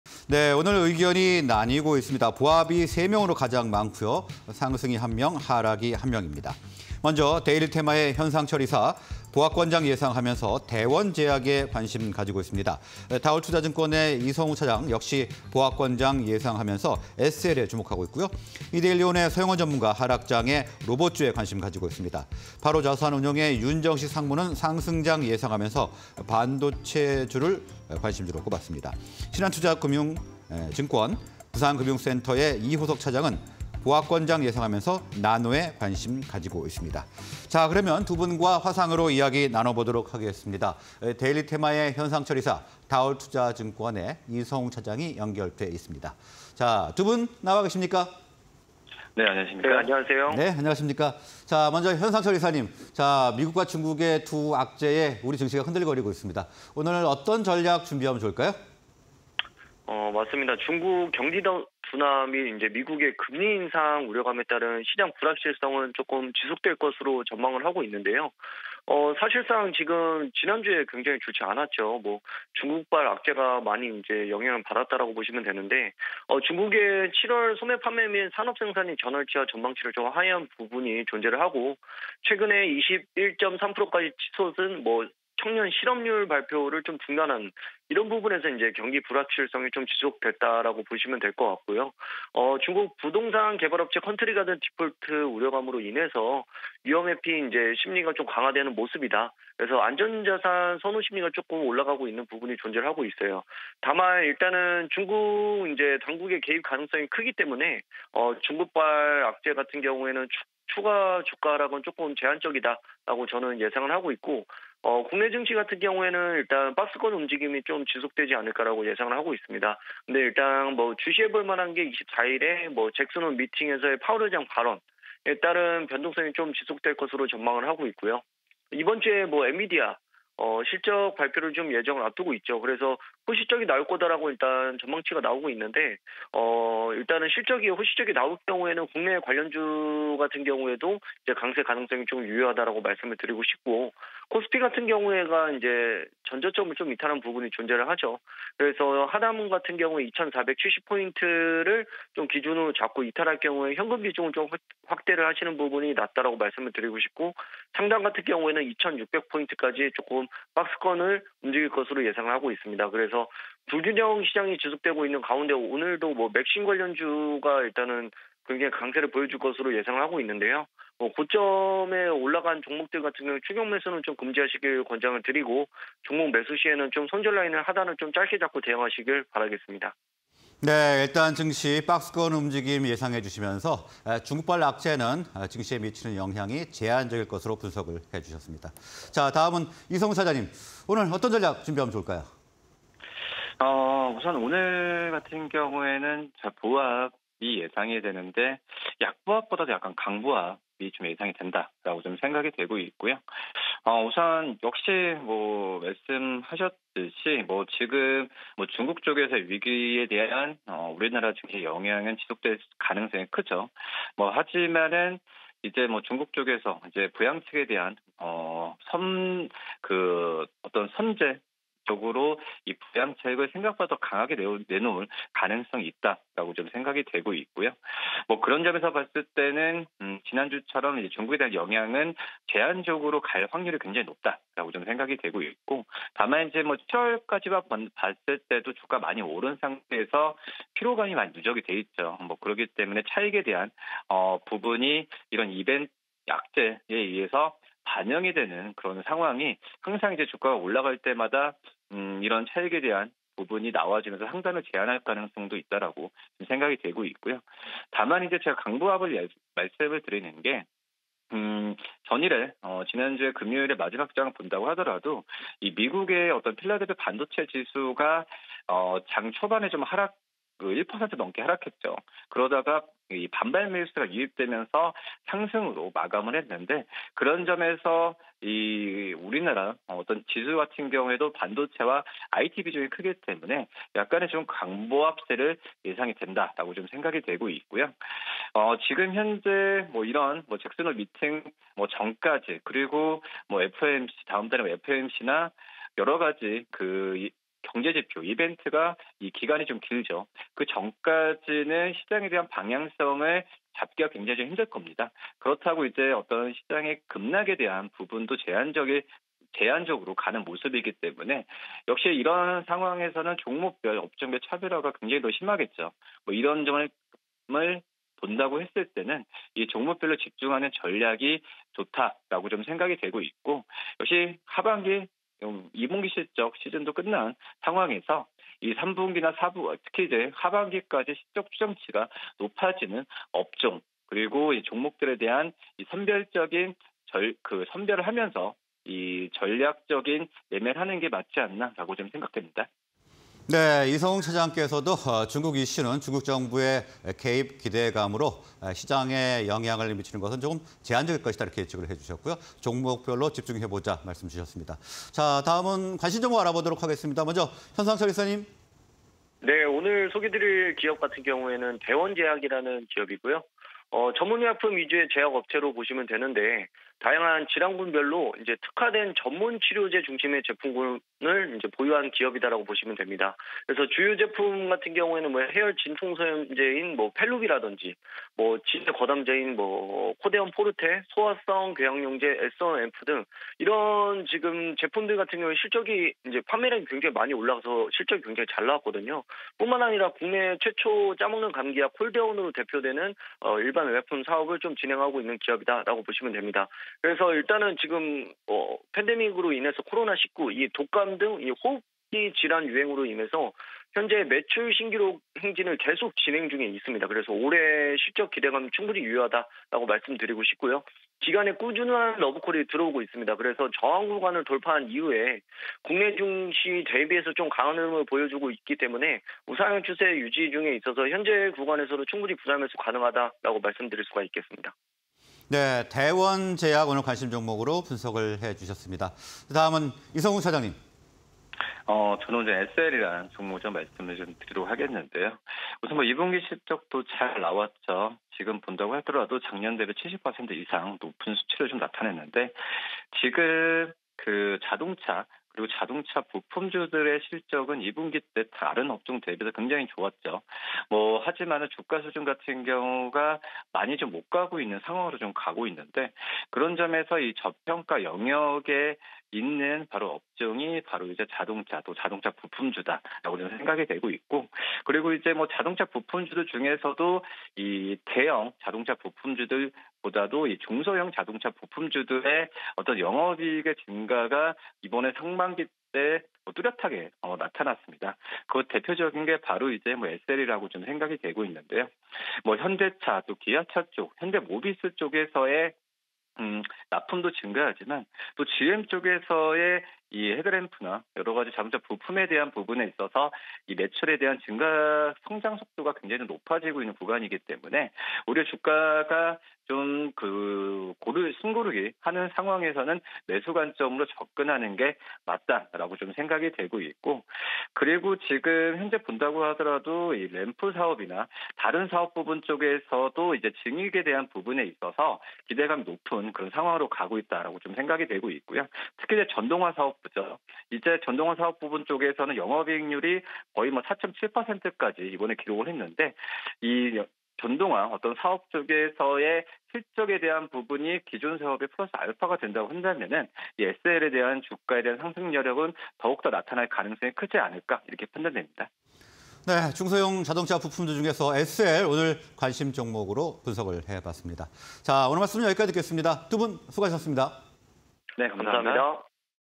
대원제약(003220)… 호흡기질환 유행으로 매출 신기록 / 에스엘(005850)… GM향 매출 증가 추세_UP&DOWN 5인의 투자 전략 (20230821)
Q. 美-中 펀치에 '흔들'… 박스피 지속될까?
Q. 커지는 중국發 우려… 국내 증시 영향은?

대원제약(003220)
- 전문의약품 중심 제약 업체
- 짜먹는 감기약 '콜대원' 대표제품
- 호흡기질환 유행으로 매출 신기록
- 올해 실적 기대감 유효
- 기관 매수… 저항구간 돌파 후 우상향

에스엘(005850)
- GM향 매출 증가 추세
- 현대차·기아 SUV 생산 비중 증가
- 증권가 목표주가 상향